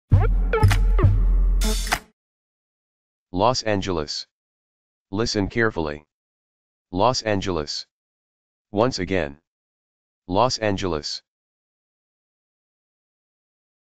Los Angeles Pronunciation ⚡️ How To Pronounce Los Angeles!
👂How to pronounce Los Angeles properly in this video!

Do you want to improve your English pronunciation? Well, you've come to the right place.

In this video you can learn how to pronounce Los Angeles.
Improve your English pronunciation with our videos and talk like a North American.

👉Learn how to pronounce Los Angeles immediately!